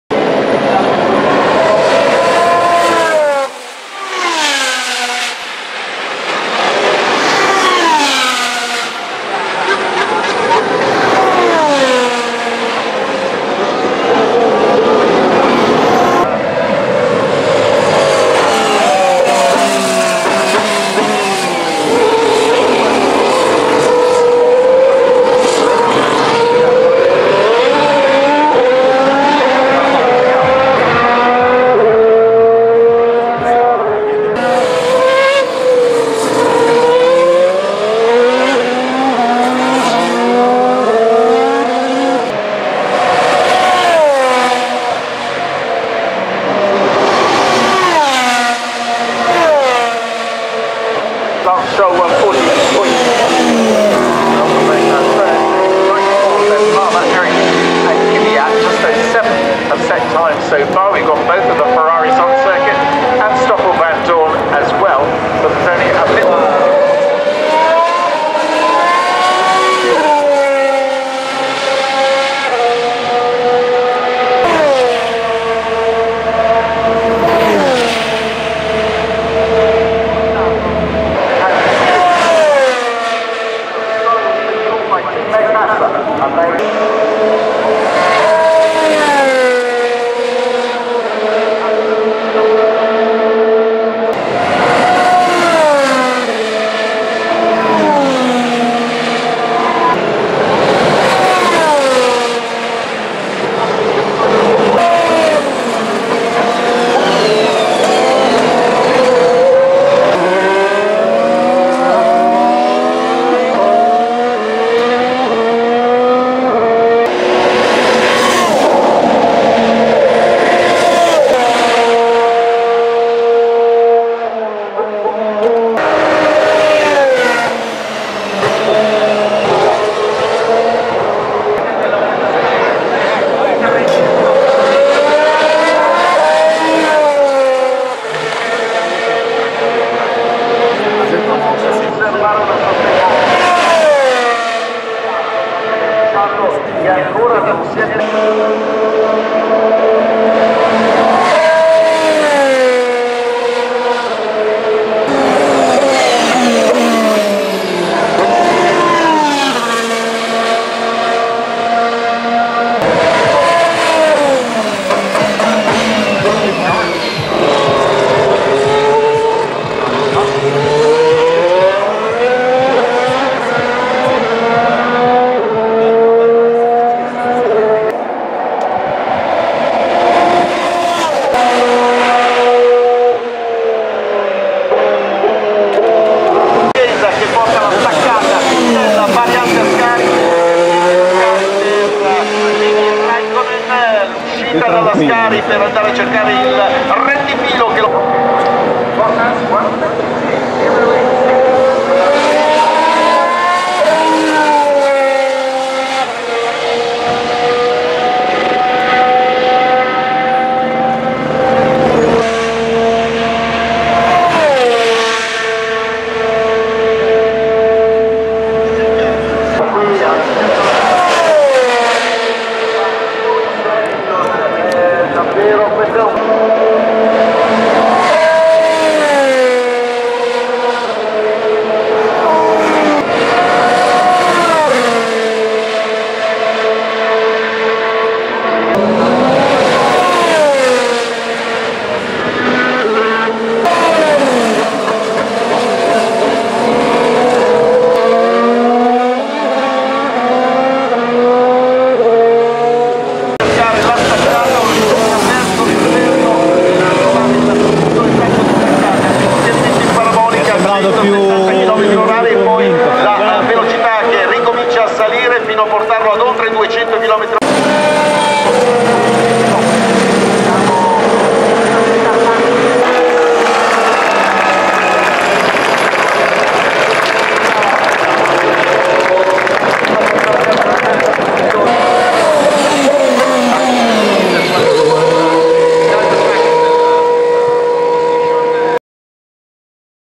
2017 Ferrari SF70H F1 car in action at Monza circuit-Vettel & Raikkonen at the Italian GP
During the 2017 Italian GP I've recorded the two Ferrari SF70H driven by Sebastian Vettel(5) and Kimi Raikkonen(7)